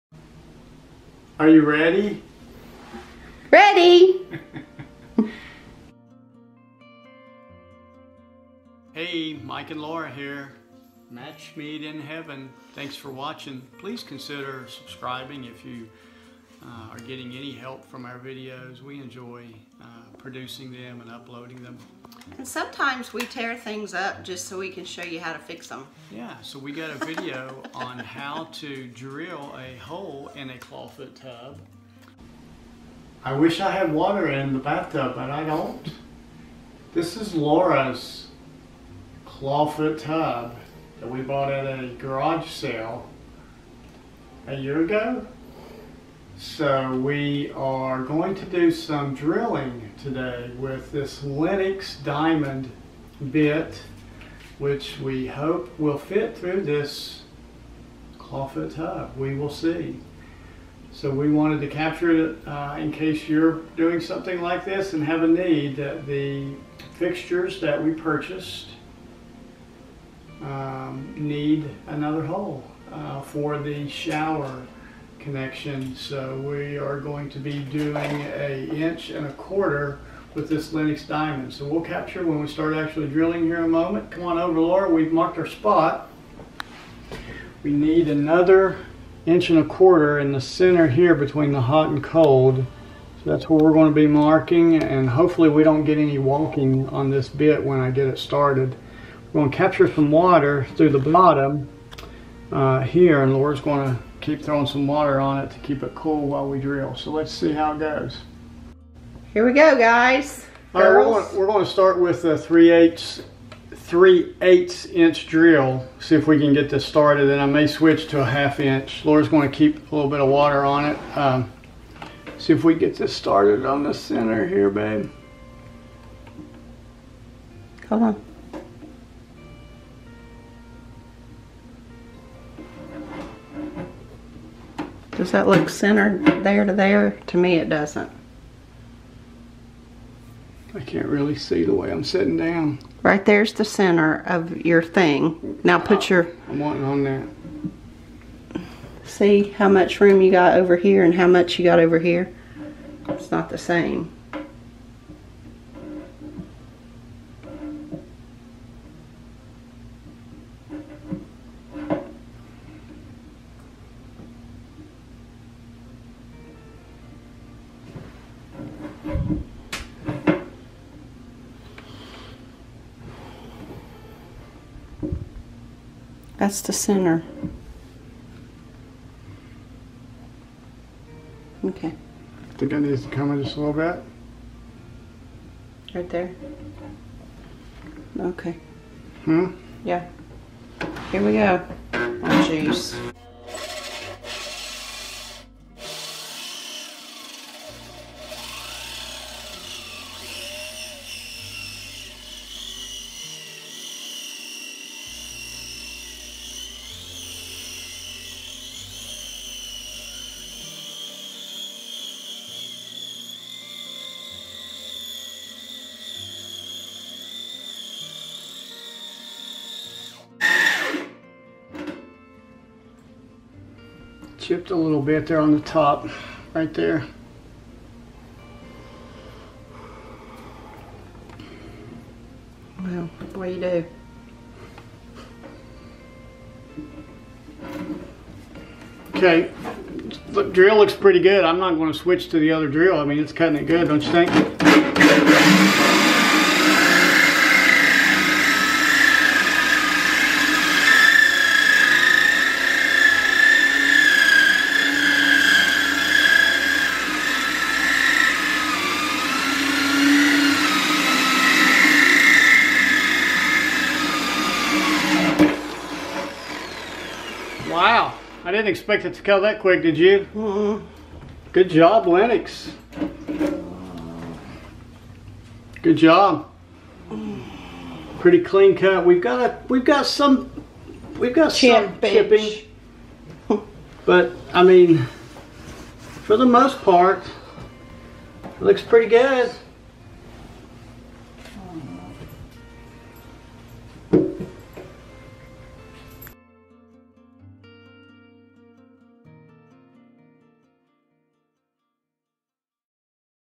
How to drill into a Antique Claw-Foot Tub - DIY & Save $
About a year ago we purchased at a yard side Laura's dream antique claw-foot tub. Then we purchased fixtures and started our masterbath remodel. Then we tried to connect our fixtures and determined we need to drill a 1-1/4" hole for them to fit. We went with Lenox Diamond drill bit and we are glad we did